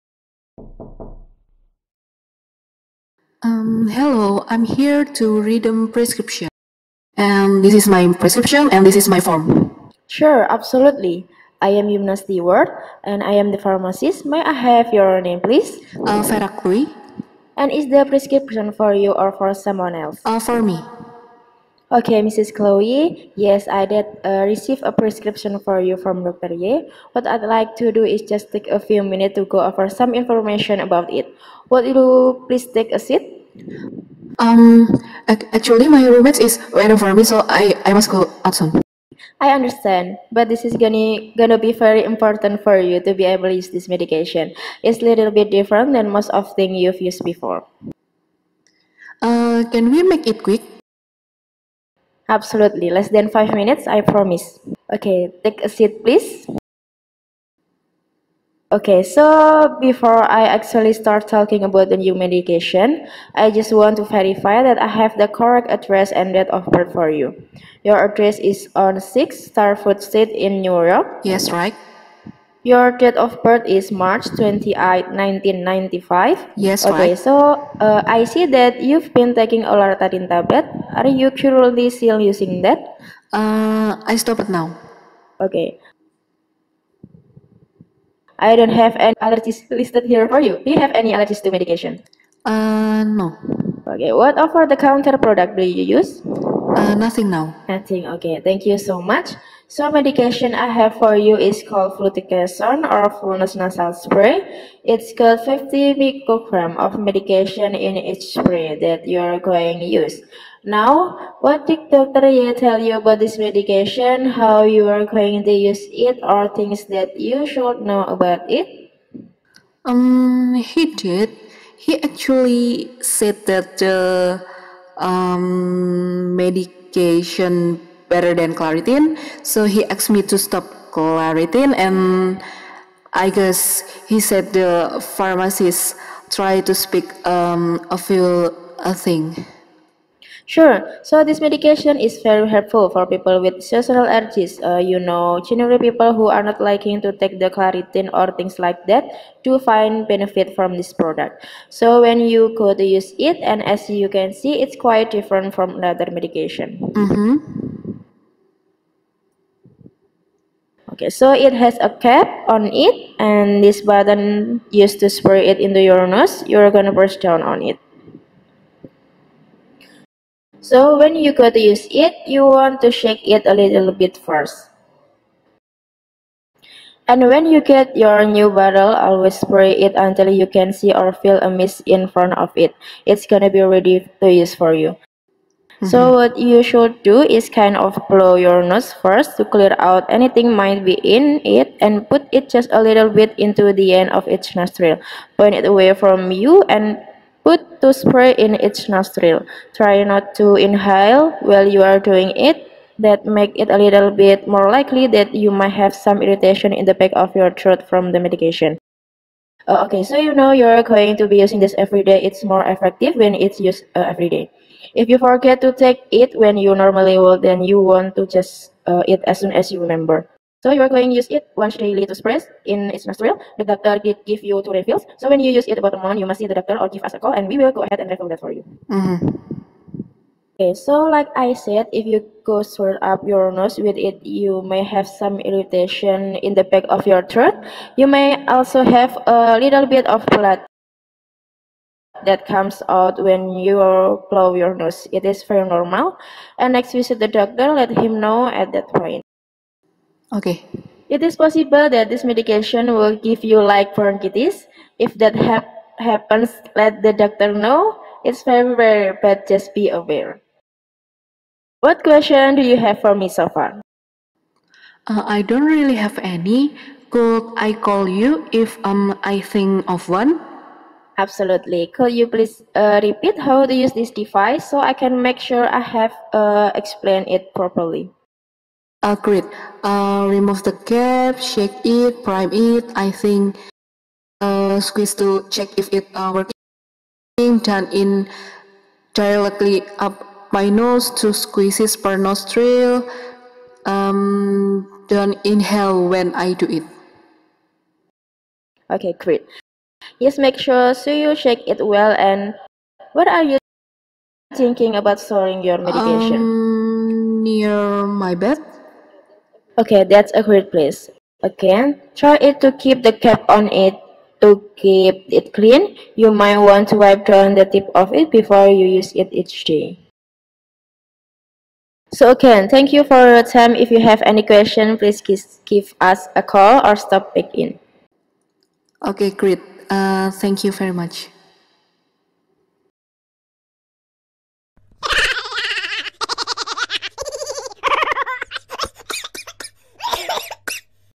Allergic Rhinitis | Fluticasone nasalspray is not unsual spray?
Hello friends! :) I hope you enjoy the pharmacist counseling simulation about  Allergic Rhinitis

Allergic rhinitis also named as “hay fever,” especially when caused by seasonal allergens. Hay fever shares many of the same symptoms as a common cold, but is not caused by a virus or bacteria. Instead, it is caused by your immune system reacting to allergens you breath into your body (acaai.org, 2019)

This video is directed, edited, simulated by:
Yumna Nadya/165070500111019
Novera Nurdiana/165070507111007

Faculty of Medicine, Departement of Pharmacy, University of Brawijaya